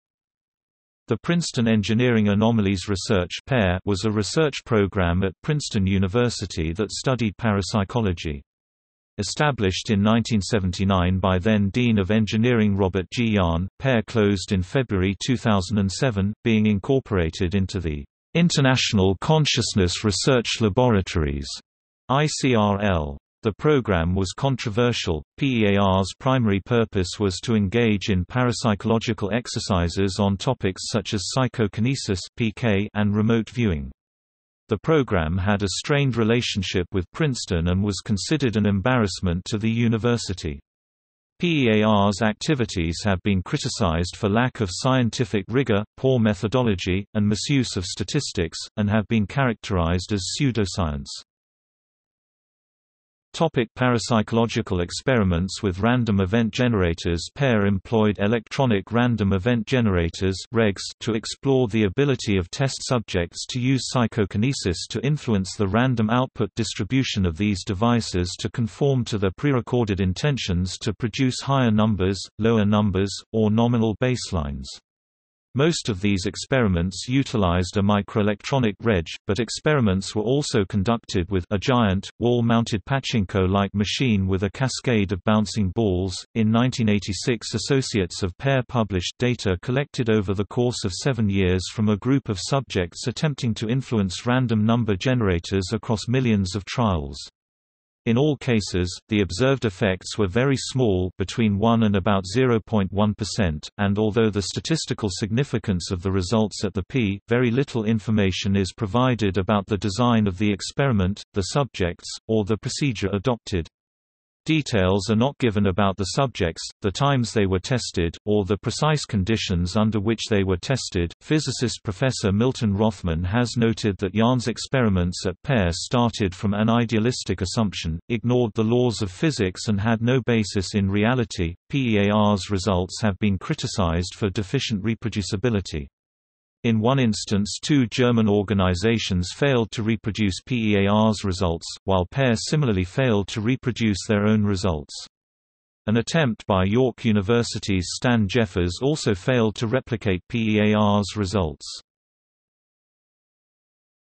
Princeton Engineering Anomalies Research Lab | Wikipedia audio article
00:00:53 Parapsychological experiments with random event generators

- Socrates

SUMMARY
The Princeton Engineering Anomalies Research (PEAR) was a research program at Princeton University that studied parapsychology. Established in 1979 by then Dean of Engineering Robert G. Jahn, PEAR closed in February 2007, being incorporated into the "International Consciousness Research Laboratories" (ICRL). The program was controversial.PEAR's primary purpose was to engage in parapsychological exercises on topics such as psychokinesis (PK) and remote viewing.  The program had a strained relationship with Princeton and was considered an embarrassment to the university. PEAR's activities have been criticized for lack of scientific rigor, poor methodology, and misuse of statistics, and have been characterized as pseudoscience.